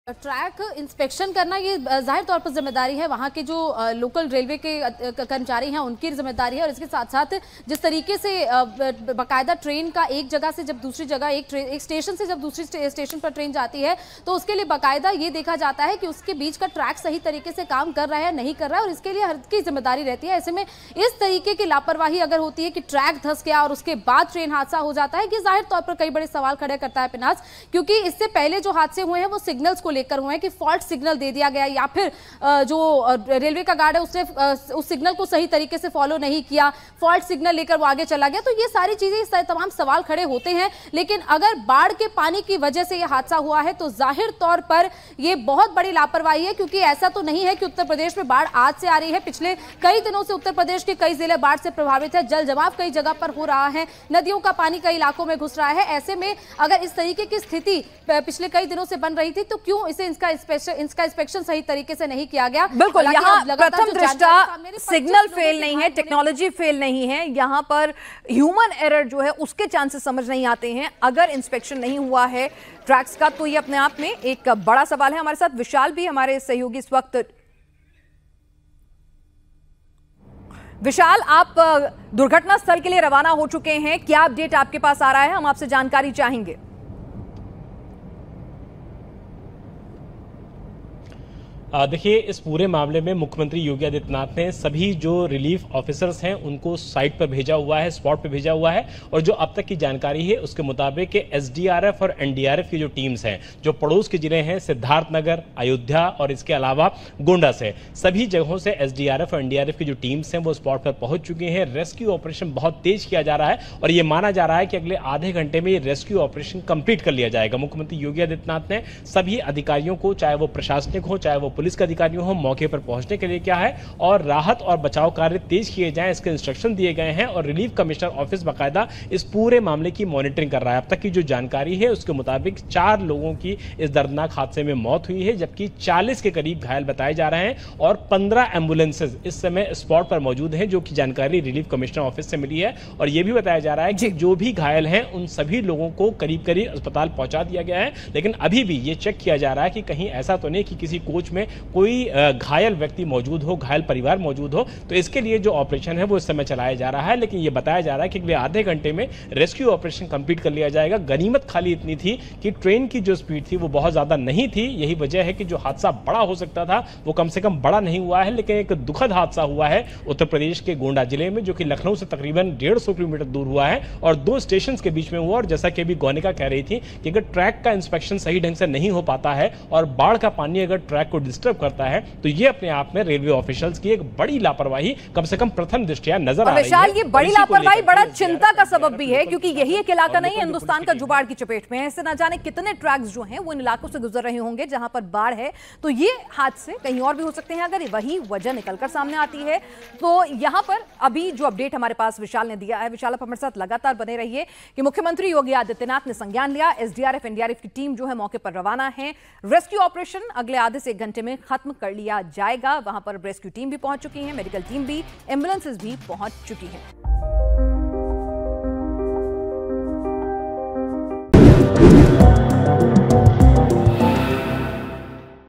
Gonda Accident: Uttar Pradesh में बेपटरी हुई Train... चार की मौत! सामने आया दिल दहलाने वाला Video
Gonda Train Accident: यूपी के गोंडा में गुरुवार को चंडीगढ़-डिब्रूगढ़ एक्सप्रेस की 5 से 6 बोग‍ियां पलट गईं। हादसे में अभी तक 4 लोगों के मौत की सूचना है। वहीं, कई लोग घायल बताए जा रहे हैं।